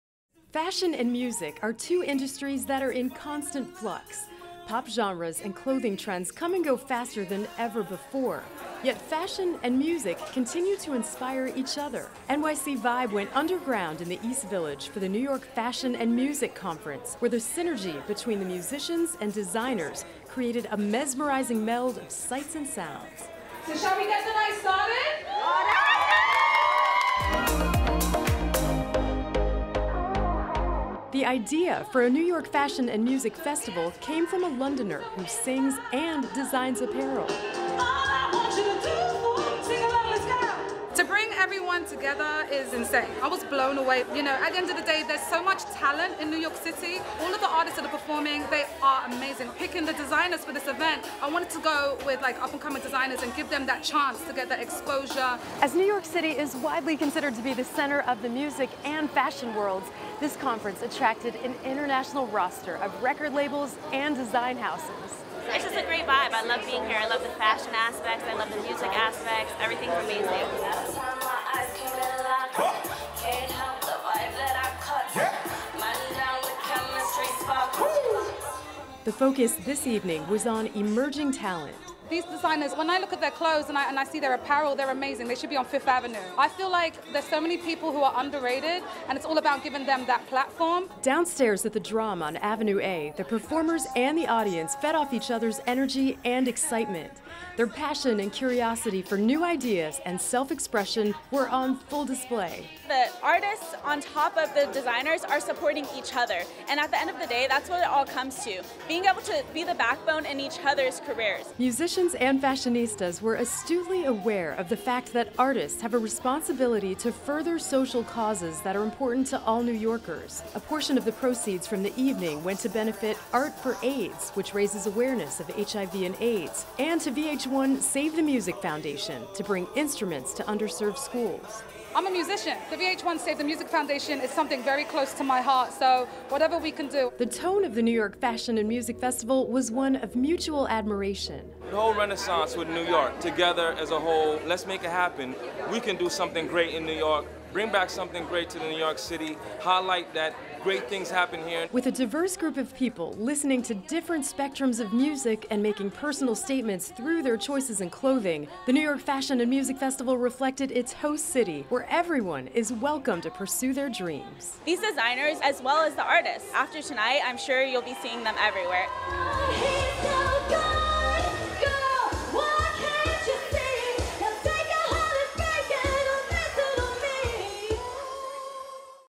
NYC VIBE: Fashion & Music Conference
Fashion and Music are two industries that are in constant flux. Pop genres and clothing trends come and go faster than ever before…yet Fashion and Music continue to inspire each other.

NYC VIBE went “underground” in the East Village for the New York Fashion and Music Conference, where the synergy between the musicians and designers created a mesmerizing meld of sights and sounds!

The idea for a “New York Fashion and Music Festival” came from a Londoner who sings and designs apparel, as New York City is widely considered to be at the center of the music and fashion worlds…

The conference attracted guest speakers from an International roster of record labels and design houses. The focus this evening was on emerging talent. The tone of the New York Fashion and Music Festival was one of mutual admiration. With a diverse group of people listening to different spectrums of music and making personal statements through their choices in clothing.

Downstairs at the Drom on Avenue A, the performers and the audience fed off each other’s energy and excitement. Their passion and curiosity for new ideas and self expression were on full display. Musicians and fashionistas were astutely aware of the fact that artists have a responsibility to further social causes that are important to all New Yorkers.

A portion of the proceeds from the evening went to benefit “Art 4 Aids” which raises awareness of HIV and AIDS, and to “VH1 Save the Music Foundation” to bring instruments to underserved schools.

The New York Fashion and Music Festival reflected its host city...where everyone is welcome to pursue their dreams!